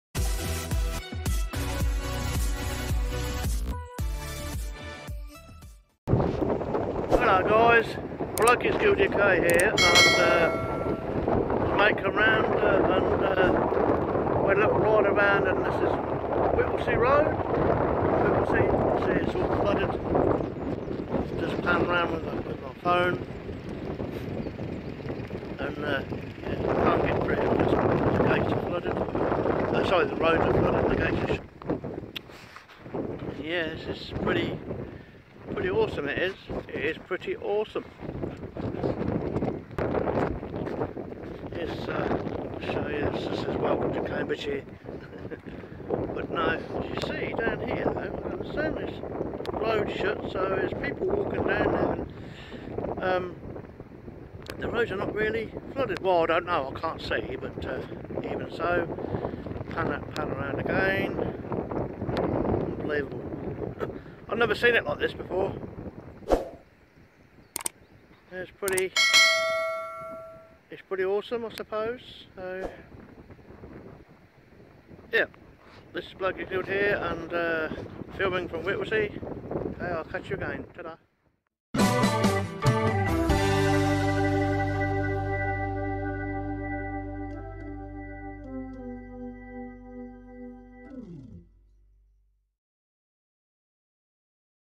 Flood warning area: B1040 Thorney to Whittlesey Road,, UK
River levels remain high in the Peterborough catchment. The Whittlesey Wash flood storage reservoir is being operated to protect nearby communities from flooding. The rising levels in the Wash will result in flooding of the B1040 road. Whilst the Flood Warning is in force, the Local Authority may close the B1040 for safety reasons. We expect the Flood Warning to be in force for several days. Diversionary routes will be put in place and further updates will be available through local media. We are closely monitoring the situation and this message will be updated by 15:00 on the 10/03/2024, or before if the situation changes.

Flood warning area: B1040 Thorney to Whittlesey Road to the South of the River Nene.